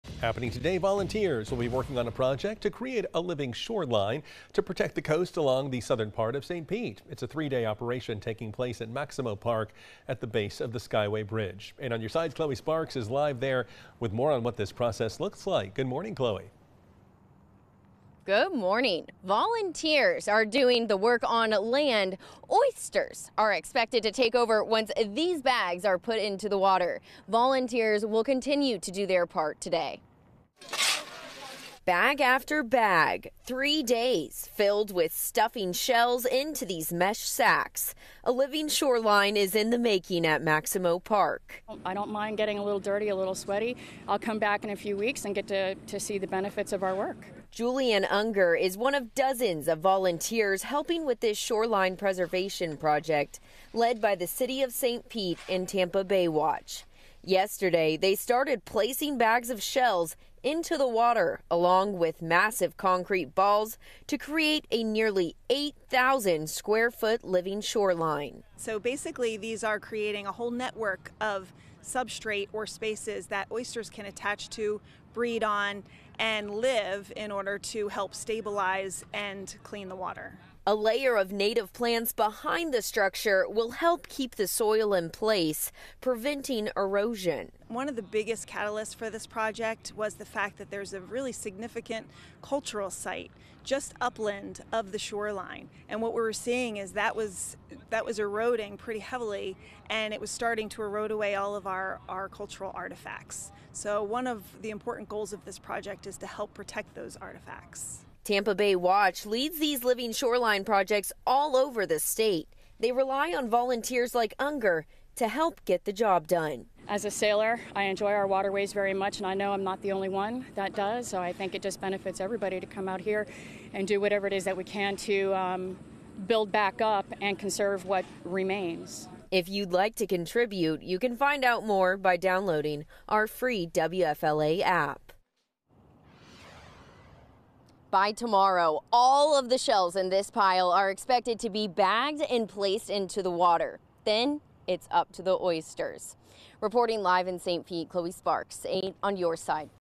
A living shoreline project underway in St. Pete
It’s a three-day operation taking place along the southern shoreline of Maximo Park, Pinellas Point Dr. S, and Sunshine Skyway Lane S.